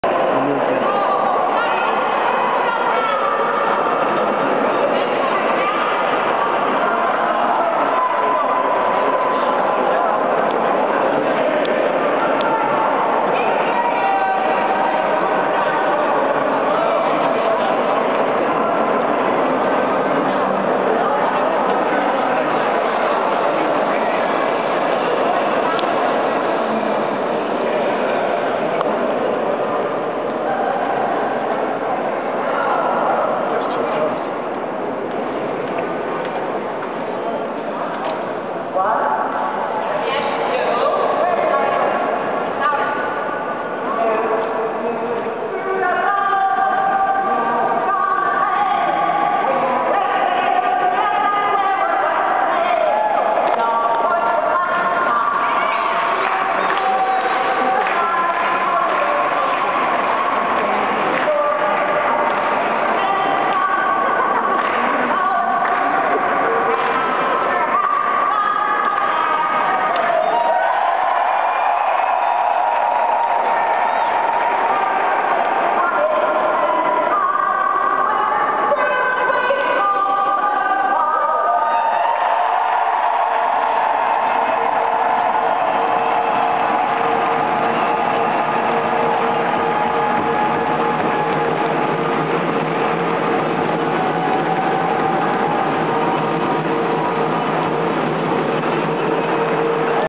Capitol Rotunda, Madison, Wisconsin, August 25, 2011 with Anne Feeney
Singing, chanting, cheering and dancing in the Capitol Rotunda on 8/25/11, part of the We Will Not Pay For Your Crisis rally.